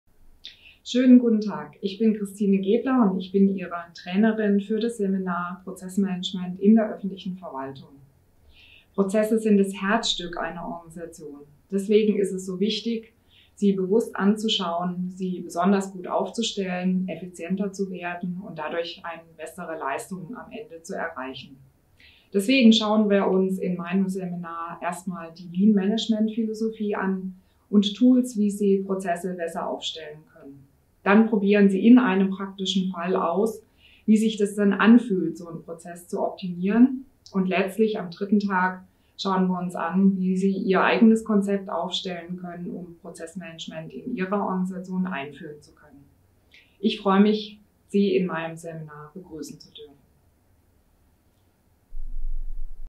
Christine Gebler - Trainerin am CETPM für Prozess- und Changemanagement
Christine Gebler ist seit 1992 Trainerin und Moderatorin und bringt über 20 Jahre Erfahrungen mit Total Quality Management (EFQM/CAF), Lean Administration, Prozessmanagement  und KVP mit. In einer großen Stadtverwaltung mit fast 8.000 Beschäftigten koordinierte sie den Kontinuierlichen Verbesserungsprozess. Sie war zudem tätig als Senior Assessorin für den Ludwig-Erhard-Preis und qualifizierte sich weiter zum Facilitator in Integrative Leadership, zur Systemaufstellerin und zum Business-Coach.

Seminare von Christine Gebler am CETPM:
Pzoessmanagement in der öffentlichen Verwaltung
Expert debriefing - Wissen sichern, bevor es verloren geht.